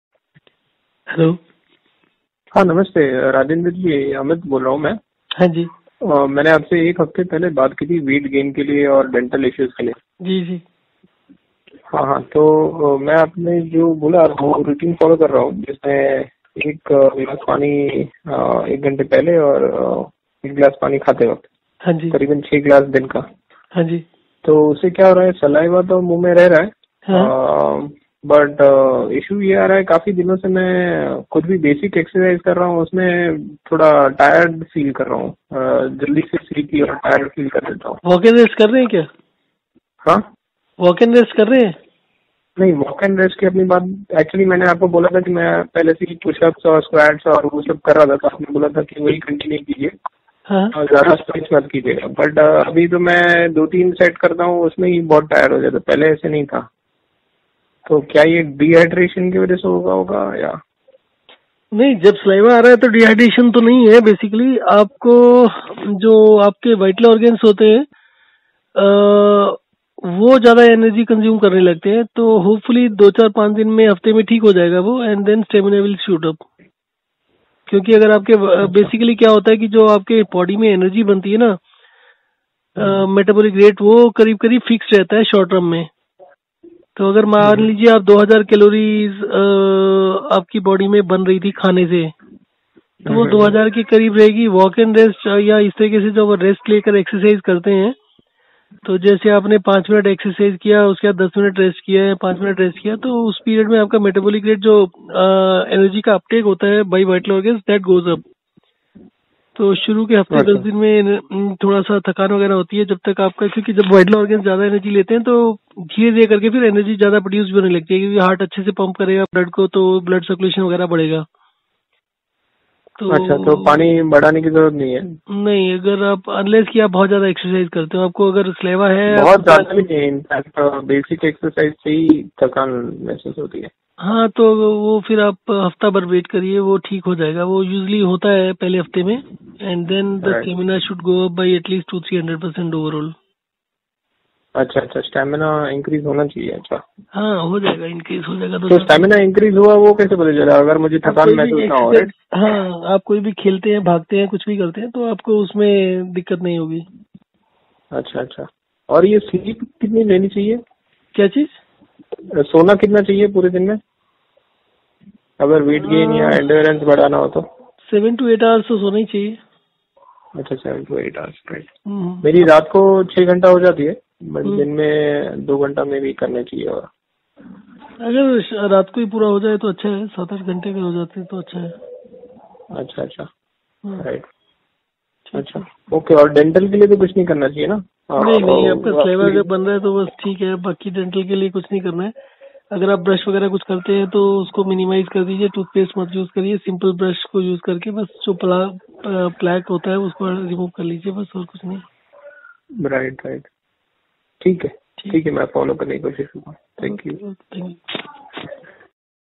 Feedback from Patient who wants to gain weight and who had Gum Swelling & Pain - Health00729-4
One Cure for All Diseases works for every disease on this earth.